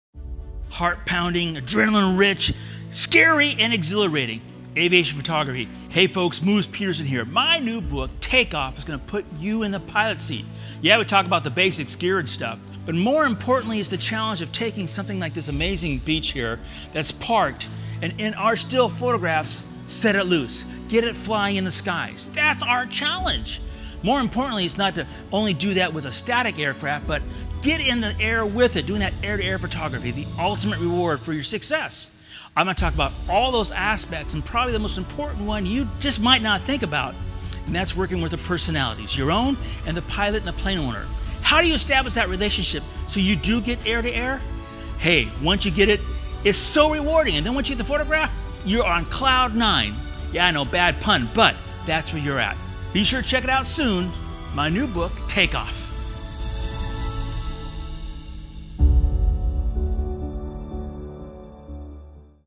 Moose Peterson's new book "Takeoff: The Alpha to Zulu of Aviation Photography"
This is a one-of-a kind book from one of the world’s most recognized experts in aviation photography. It not only teaches you exactly what you need to know to start making beautiful, intriguing photographs of aircraft, but inspires you and pushes you creatively and technically every step of the way. With this book, your camera, and your passion, your success in aviation photography will quickly take flight!